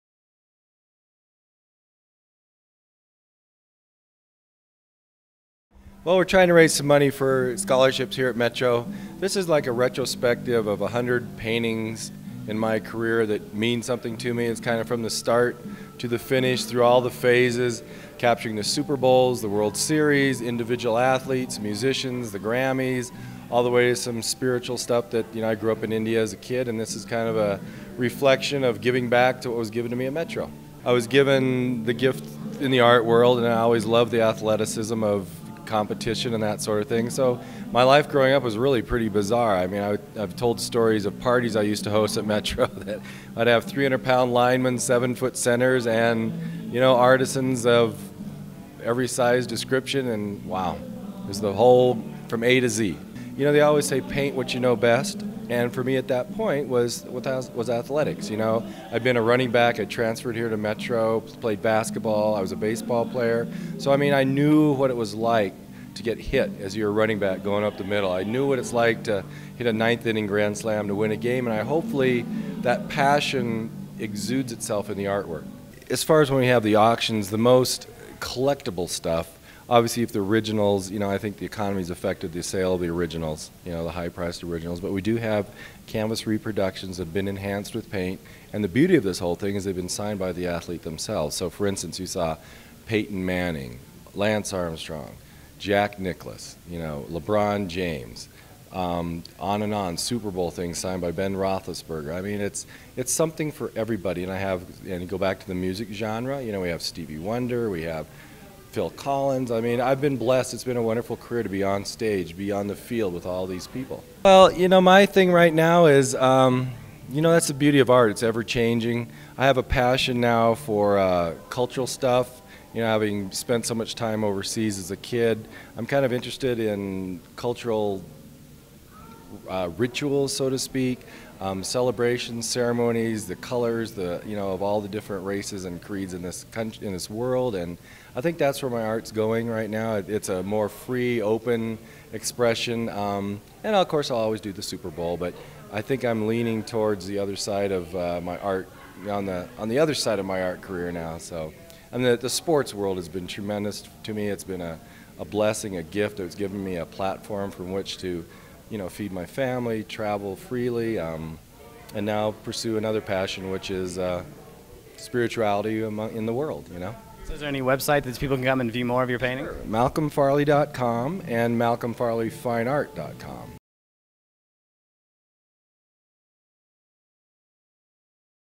MALCOLM FARLEY GALLERY.mov
Malcolm Farley had a gallery showing at the Denver Pavilions and it features over 100 bodies of work from the sports world, the religious world and even the music world. Farley talks about his inspirations and passions for painting and sports. Video by Eric Lansing